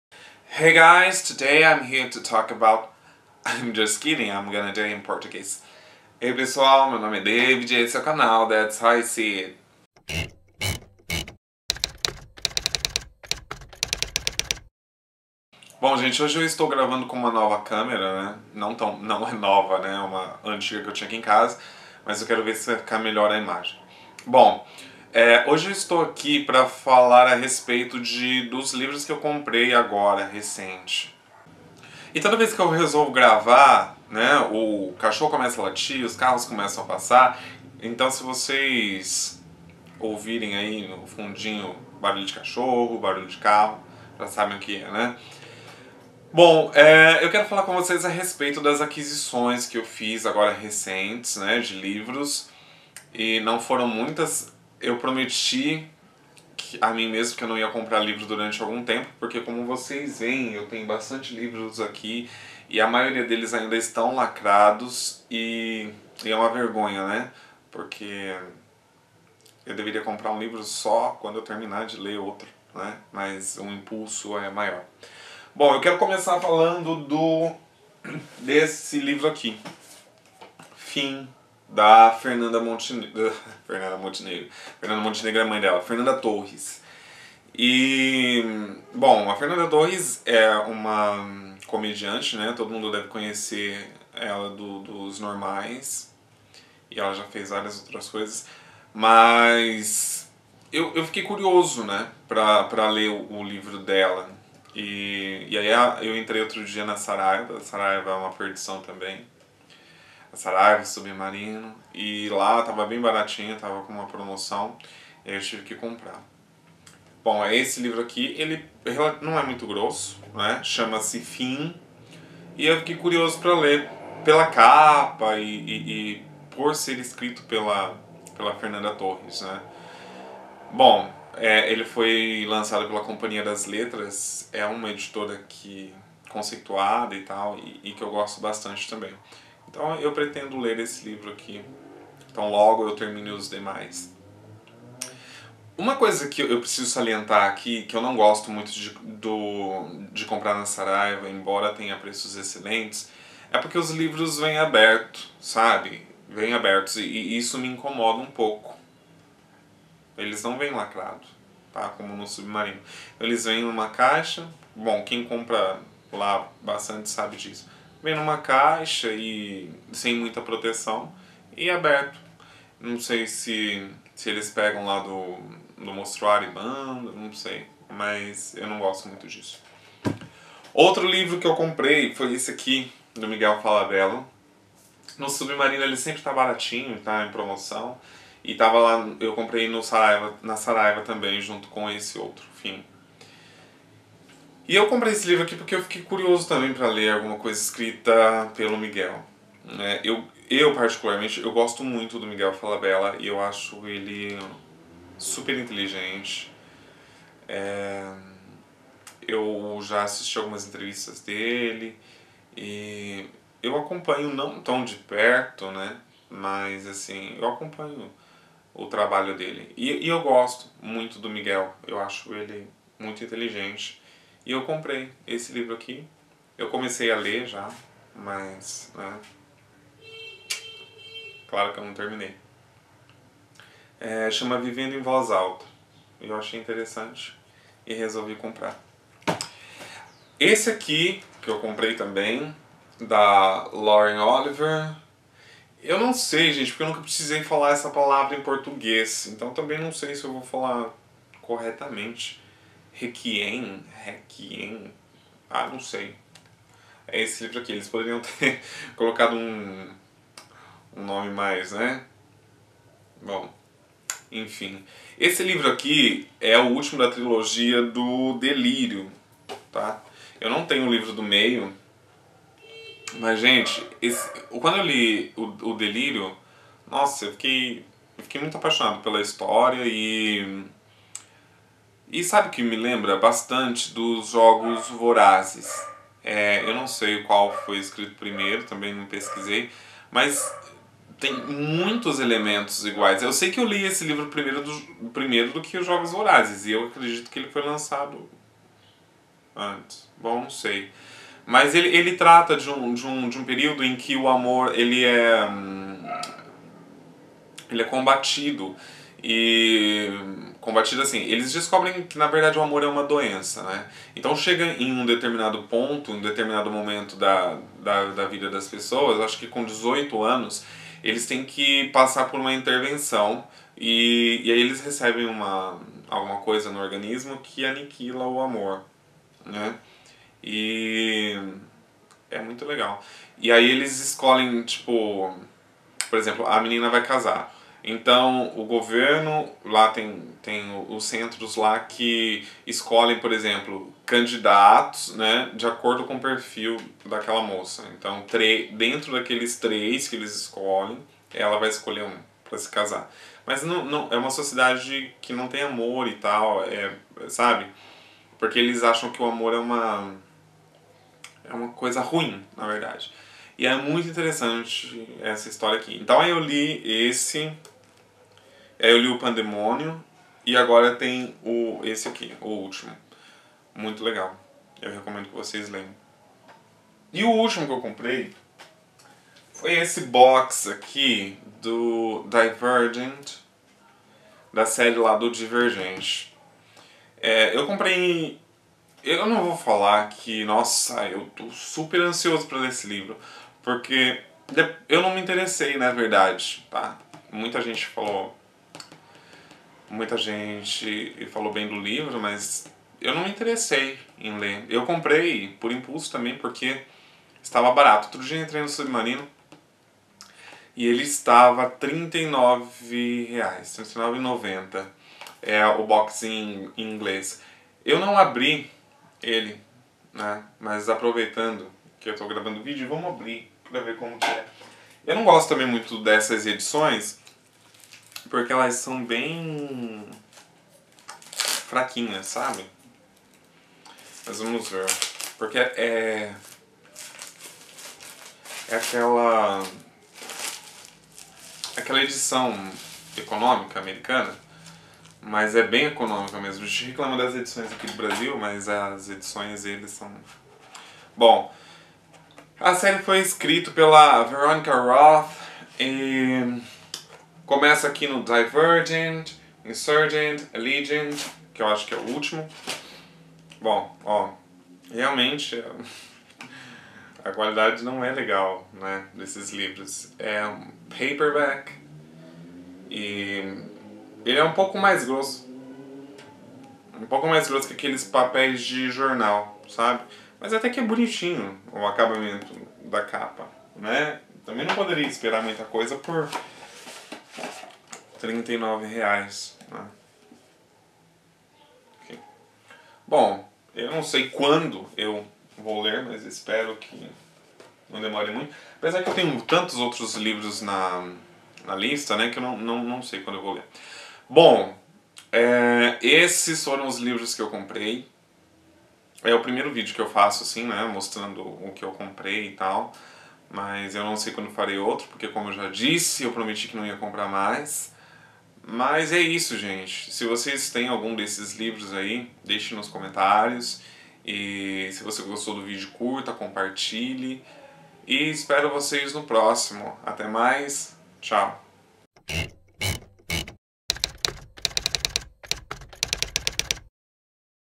O que há de novo em minha estante #1
Livros:

Réquiem - Lauren Oliver - Intrínseca
Fim - Fernanda Torres - Companhia das Letras
Vivendo em Voz Alta - Miguel Falabella - Lua de Papel
Trilogia Divergente - Veronica Roth - Harper Collins